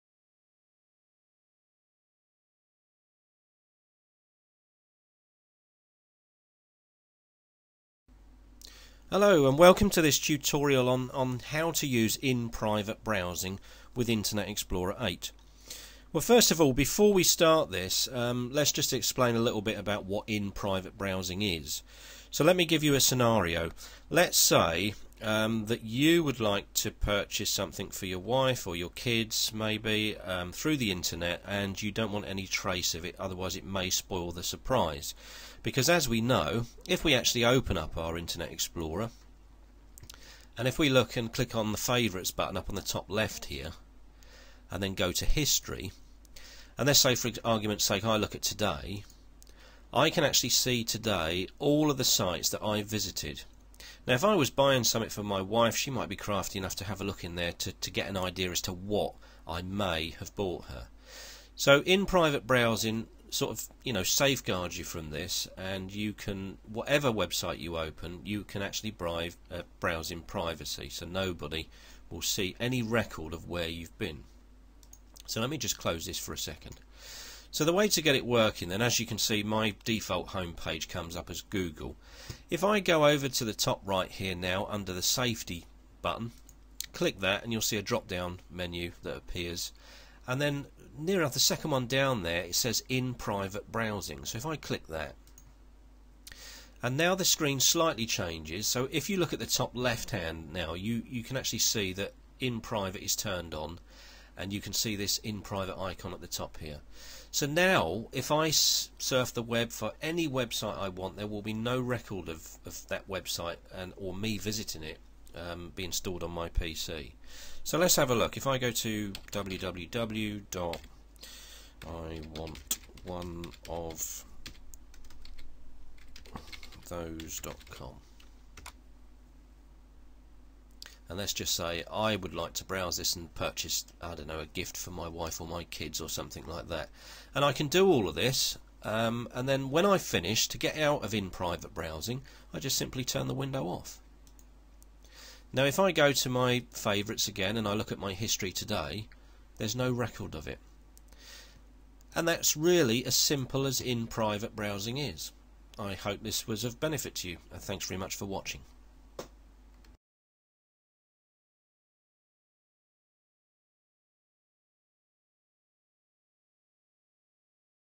How to use in private browsing on Internet Explorer 8 (Watch in HD)
to start in-private browsing for Internet Explorer 8 follow these steps:

1. Open up Internet Explorer
2. click on Safety button at the top right
3. select InPrivate Browsing

Thats it!

Whatever webpage you view now will be private and will not record trace on your PC.